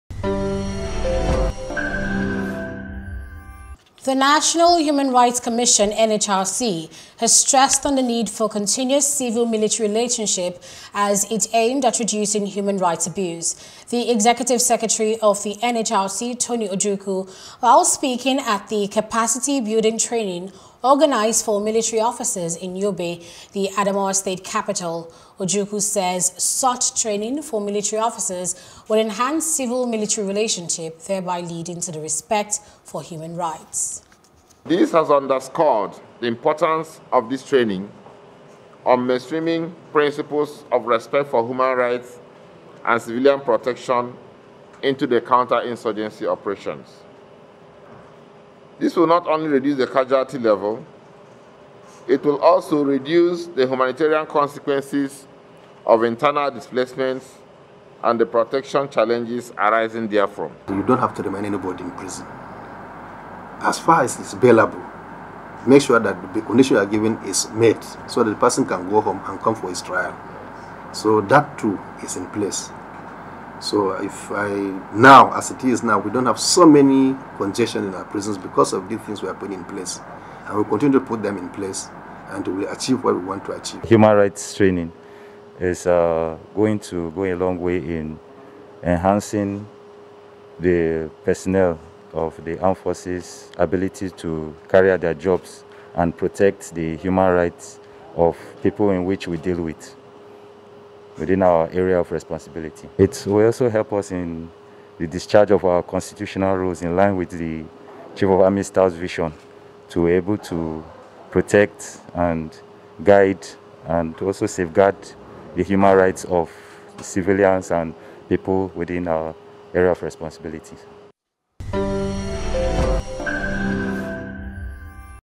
NHRC Emphasises the Need For The Protection of Citizens' Rights
The National Human Rights Commission (NHRC) has stressed on the need for a continuous civil-military relationship as it aimed at reducing human right abuse.